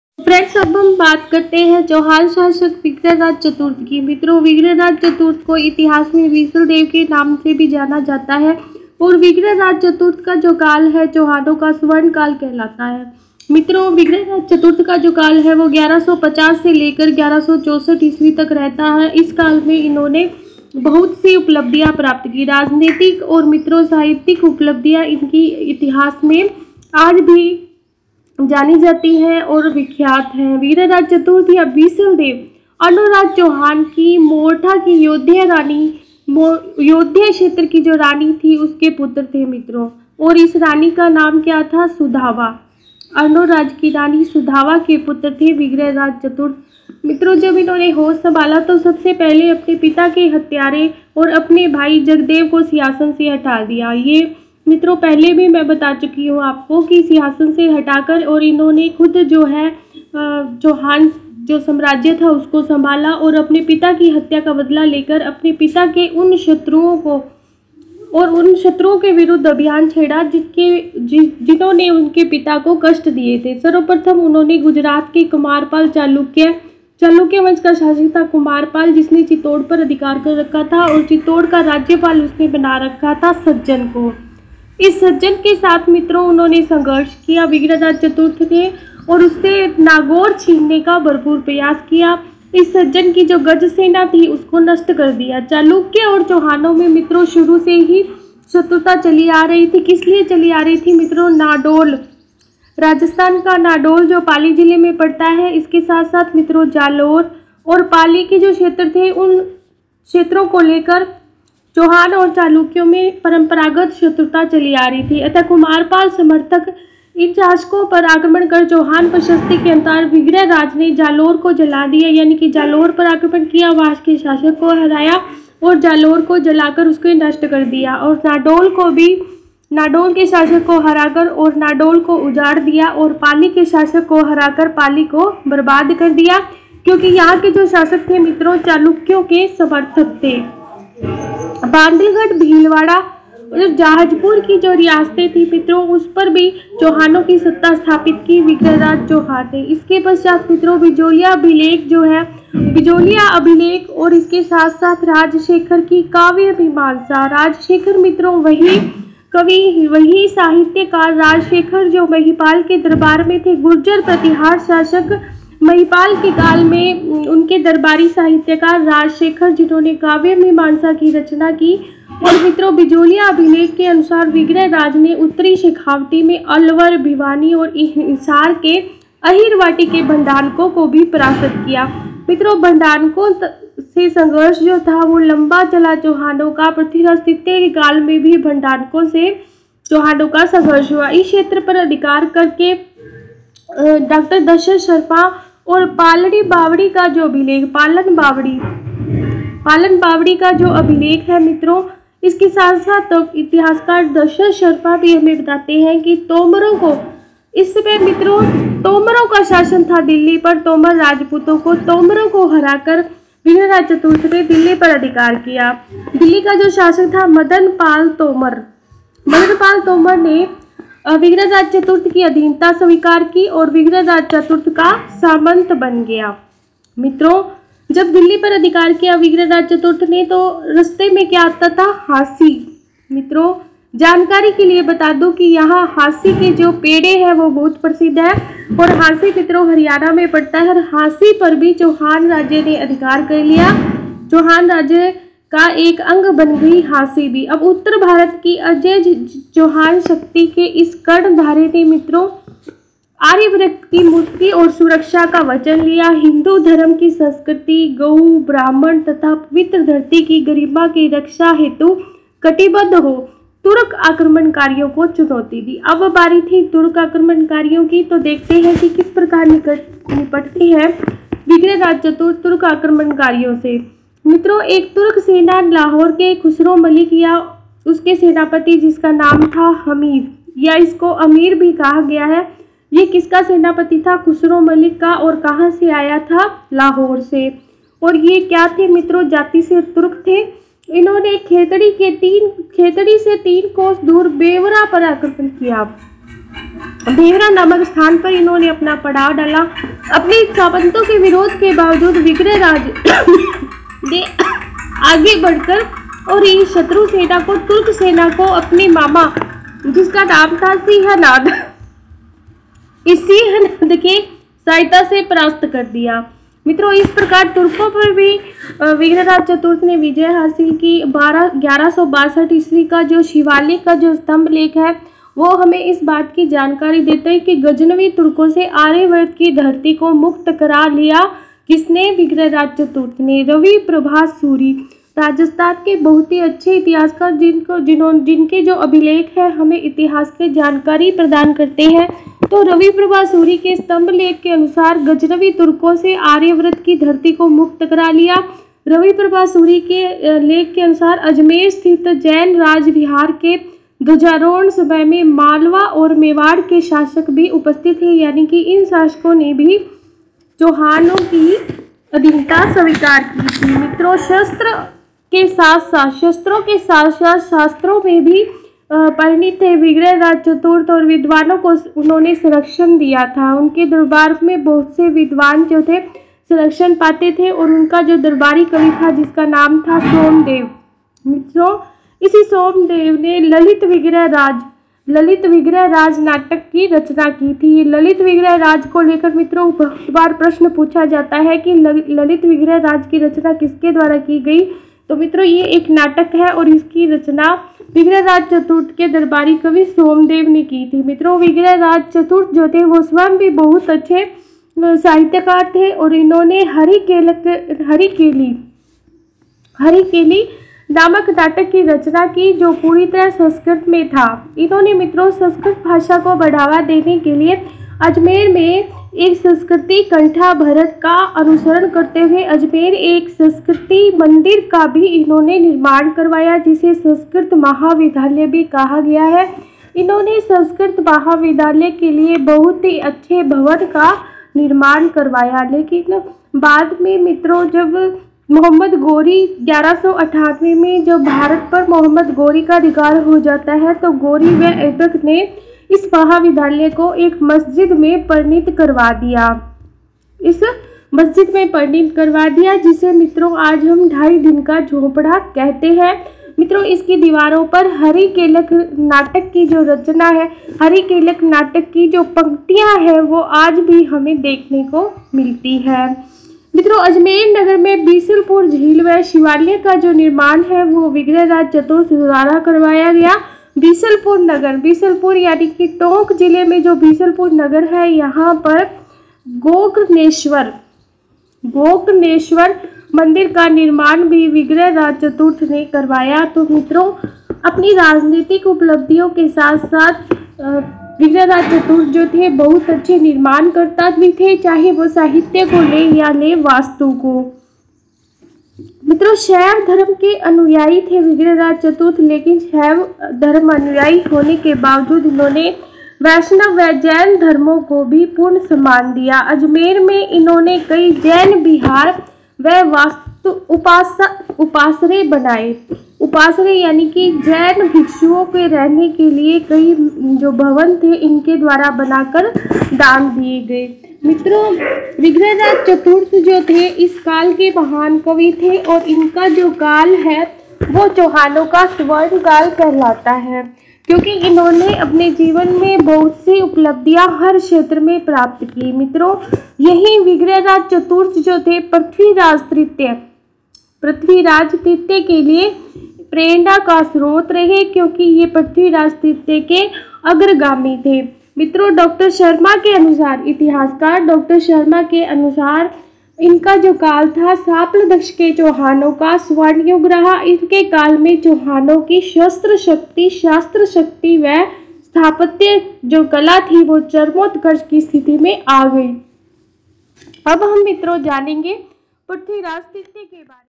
विग्रहराज चौहान का इतिहास | vigrahraj chaturth ka itihas | bisaldev chauhan ka itihas
Here we will tell you about bisaldev chauhan ka itihas in hindi kindly watch this till end.

Time stamps
0:00 Biseldev Ka itihas
2:04 Vigrah Raj Chaturth Dwara Kiye Gaye Yudh
8:28 Vigrah Raj